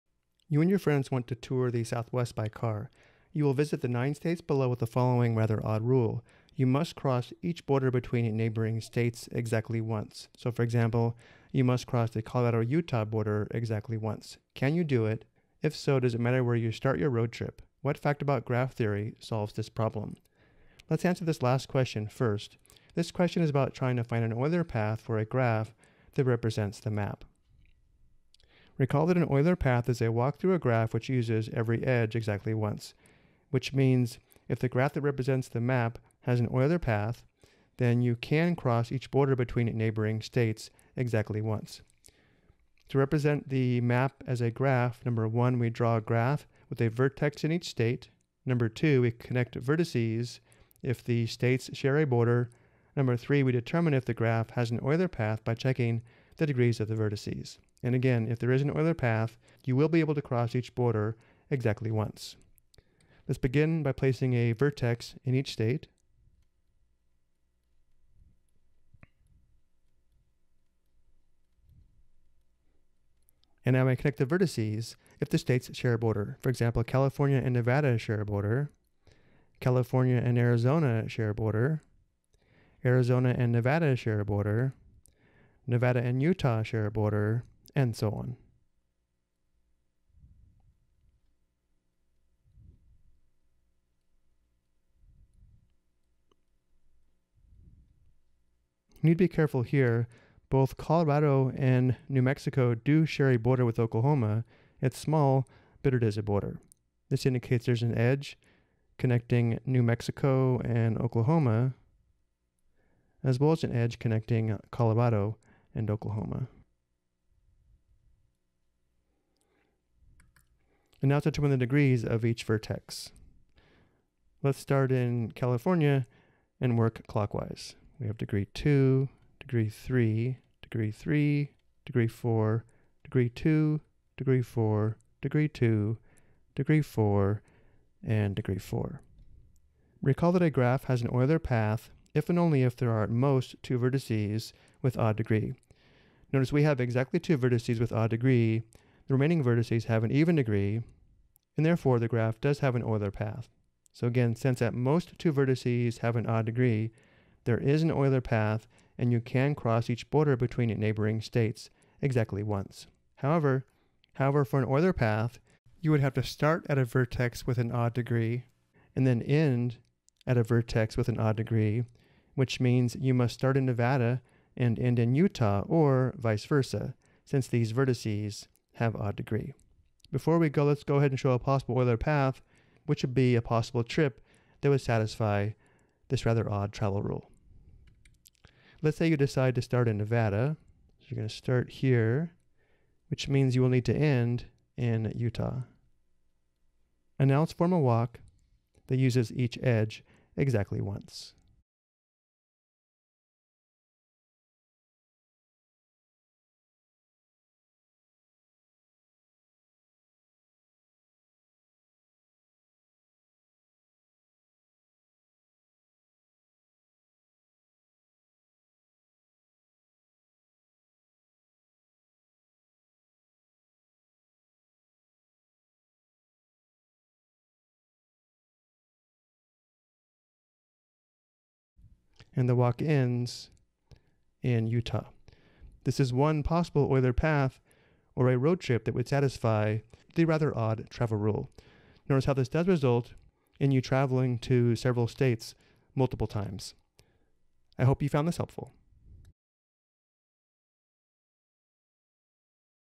Euler Path Application: Road Trip
This video provides an application of an Euler path involving a car road trip.